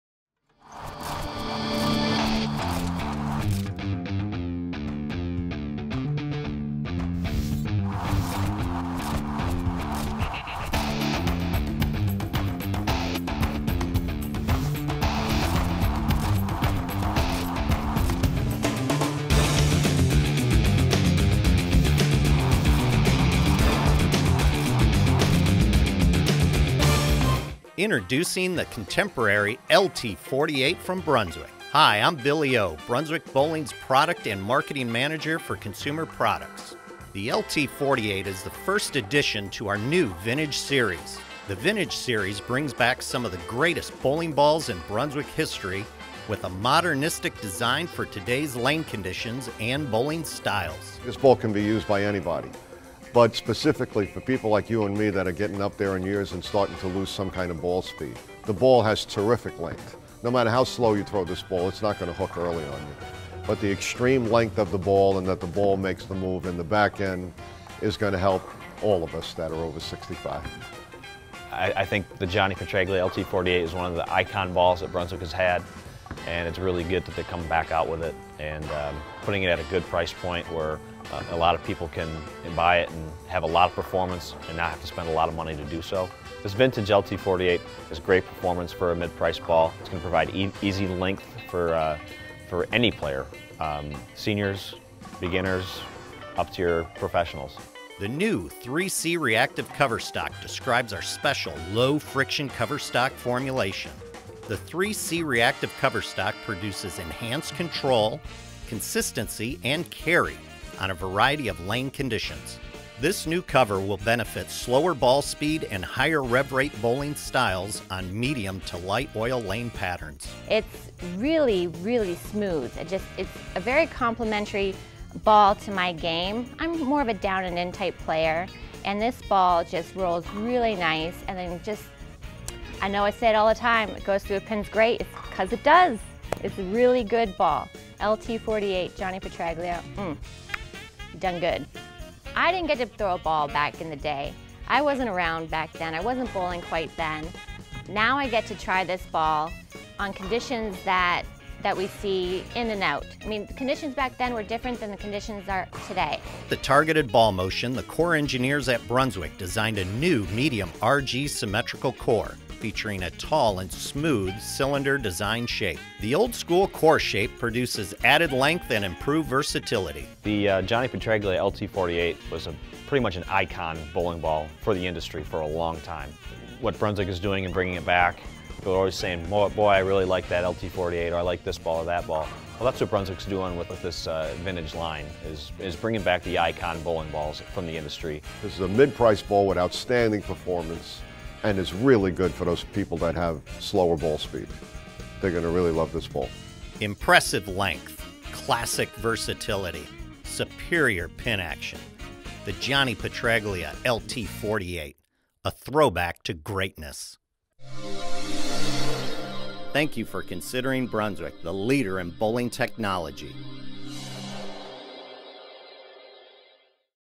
BowlersMart.com Brunswick LT-48 Bowling Ball
The Vintage LT-48 combines old-school styling with new technology to make this ball an instant classic.